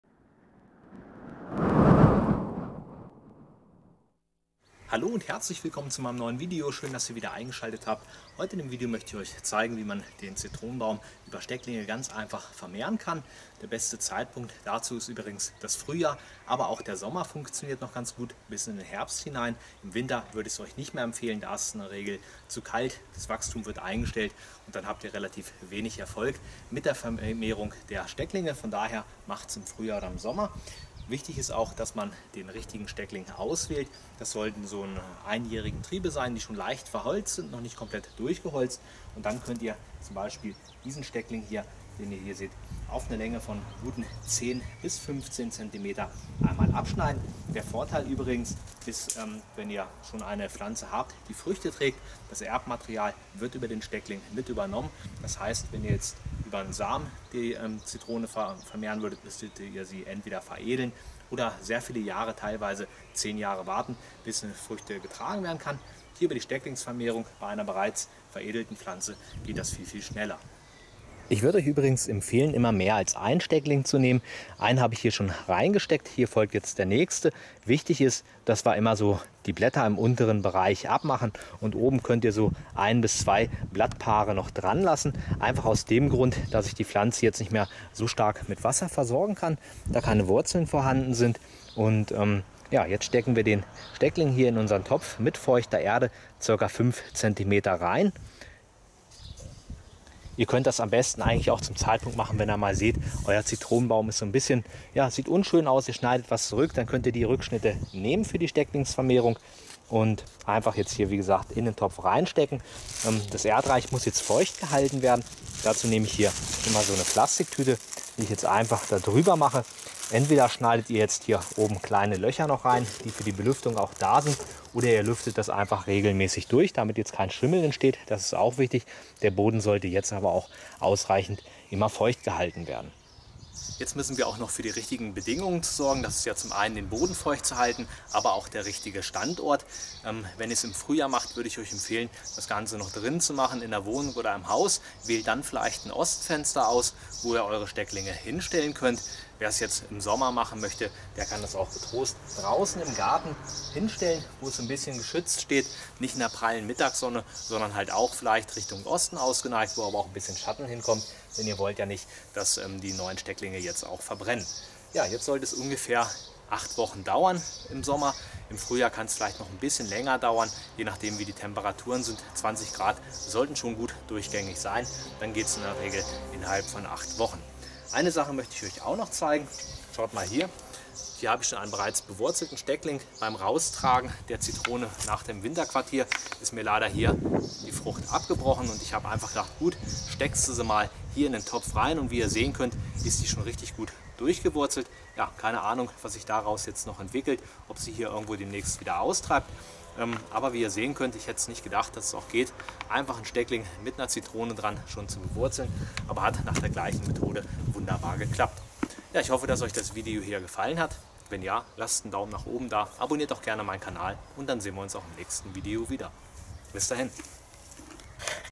Zitronenbaum vermehren durch Stecklinge Ableger Zitrone machen
Ableger von der Zitrone bzw. dem Zitronenbaum kann man einfach selbst machen und bewurzeln. Ich zeige euch, wie die Stecklingsvermehrung vom Zitronenbaum geht und wie ihr erfolgreich euern Zitronenbaum über Stecklinge vermehrt.